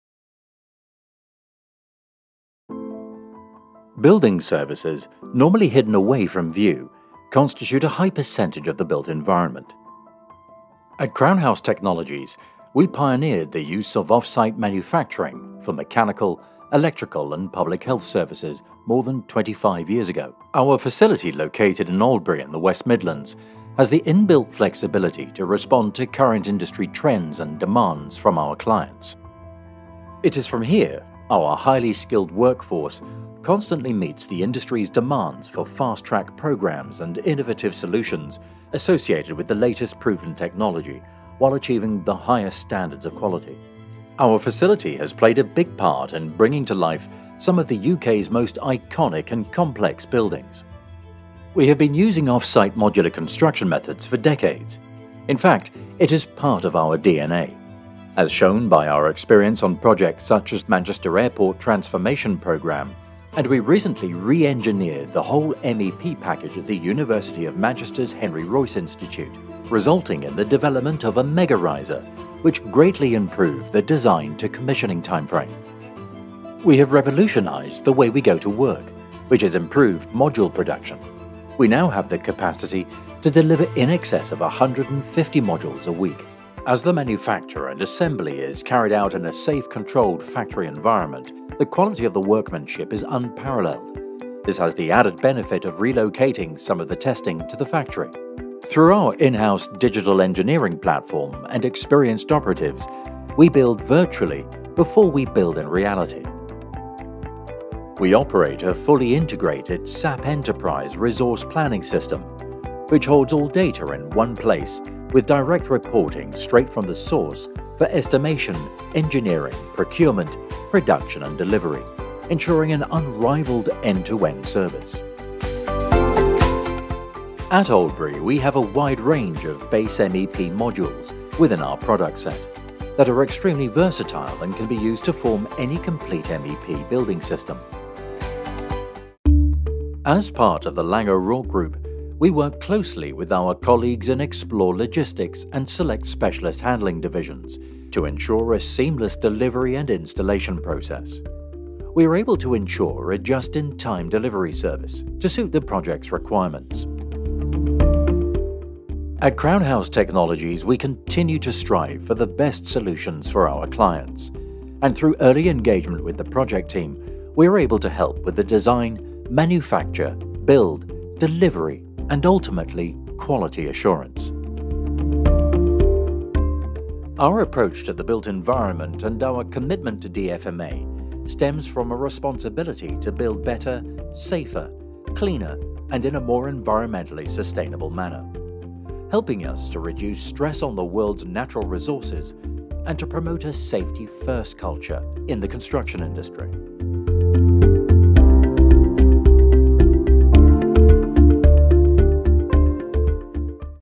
Crown House Technologies Manufacturing
At Crown House Technologies we pioneered the use of offsite manufacturing for mechanical, electrical, and public health services more than 25 years ago.

Building services that are often hidden from view, make up a large element of the built environment which is why it is crucial that they are designed and manufactured to the highest standards of quality, performing efficiently and sustainably. It’s also important that they can be delivered and installed on projects quickly, meeting fast-track programmes. Innovative offsite modular construction methods play an important role in meeting these requirements.

Our manufacturing facility in Oldbury, in the West Midlands introduced the use of offsite manufacturing for mechanical, electrical, and public health services more than 25 years ago and has not stopped innovating!

Watch our brand new video to find out why we are considered one of the UK’s most advanced and forward-thinking engineering and building services specialists, trusted to work on some of the most prestigious projects.